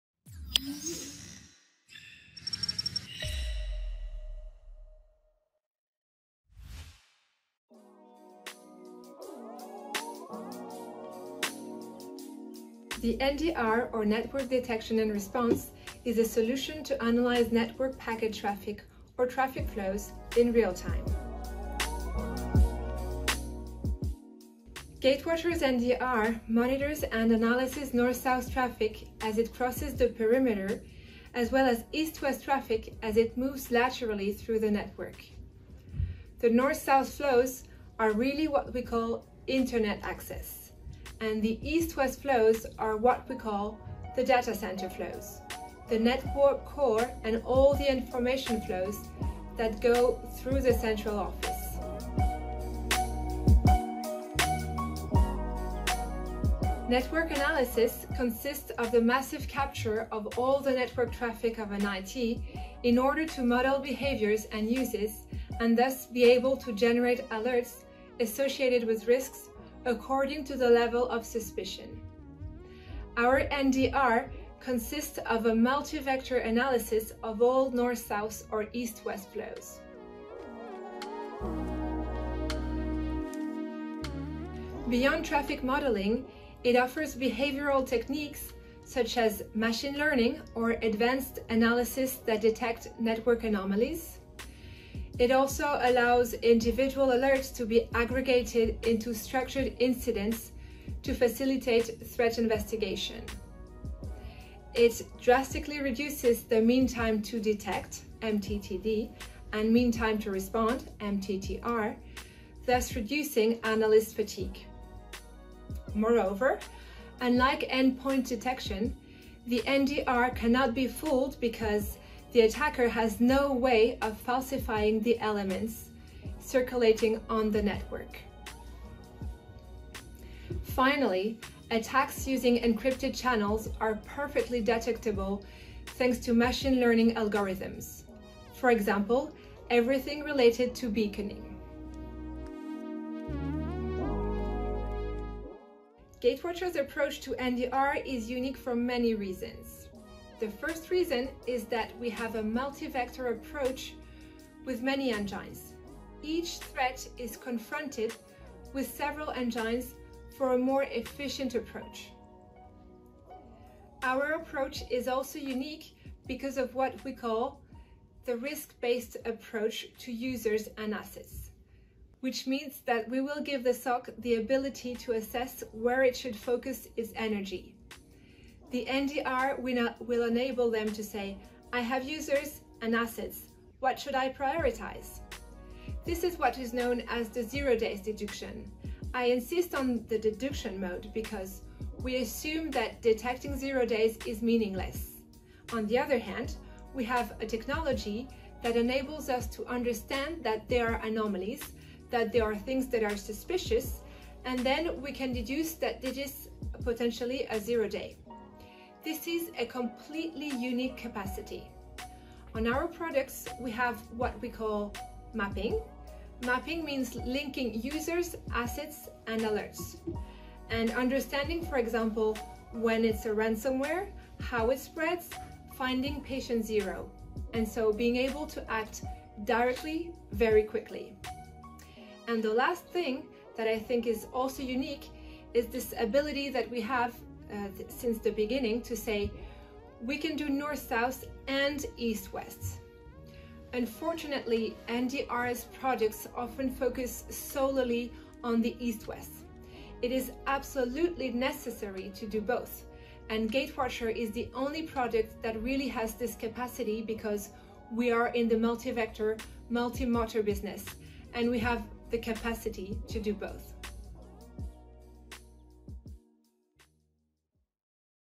Easy as NDR | What is a Network Detection and Response solution?
Download our NDR insight guide to know more about the Network Detection and Response technology.

Thanks to our "Easy as NDR" video series, take a few minutes to better understand the Network Detection and Response technology:
      - What is NDR?
      - What is network analysis?
      - What are the benefits of such a solution for companies?
      - And finally, what makes Gatewatcher's approach to NDR unique?